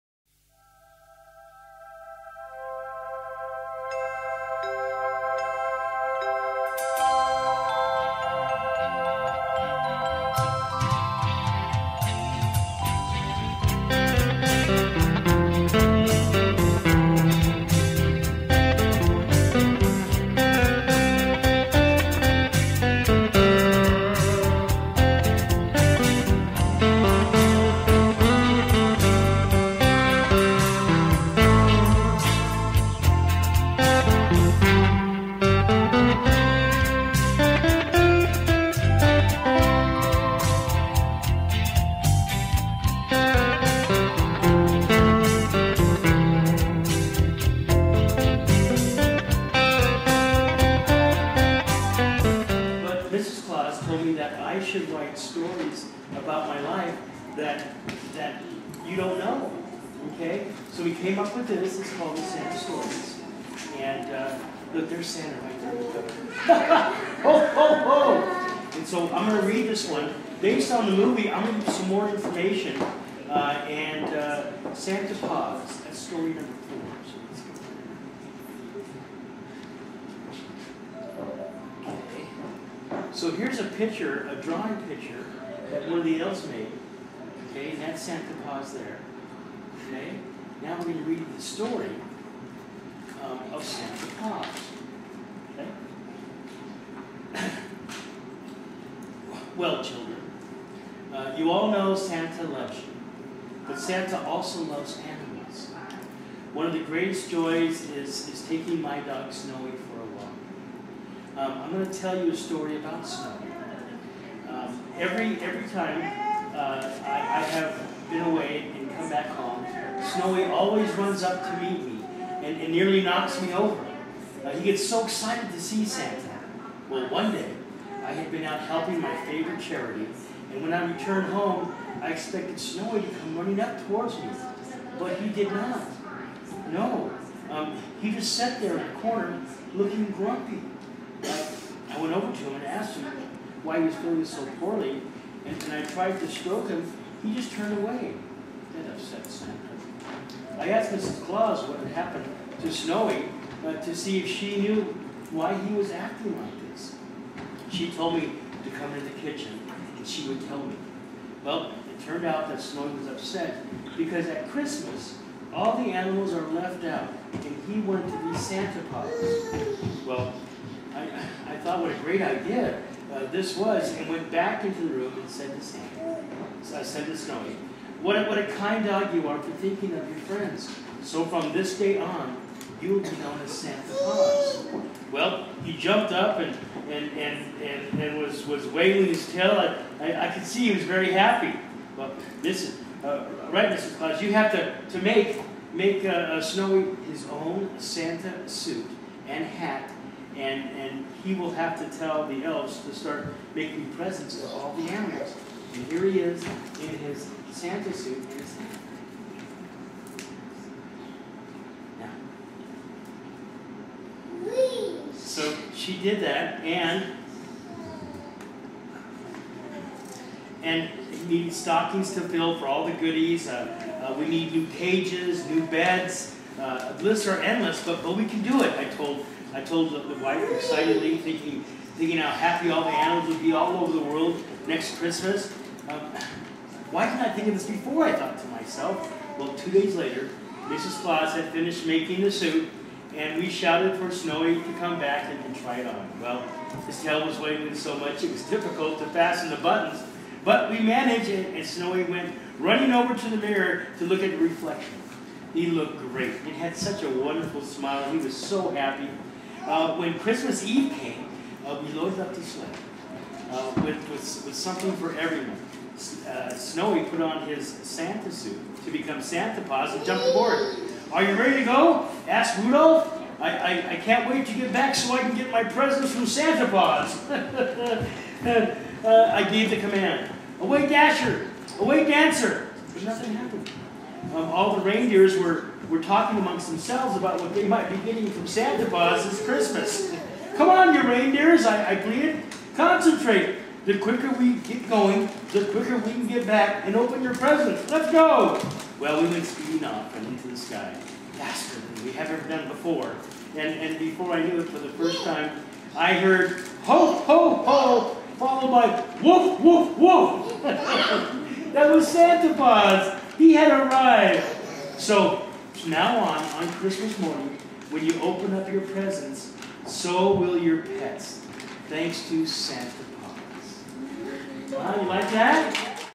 Santa at AV Dance Studio 81
Santa stops by AV Dance Studio 81 to share Christmas joy, telling stories, and taking pictures with children.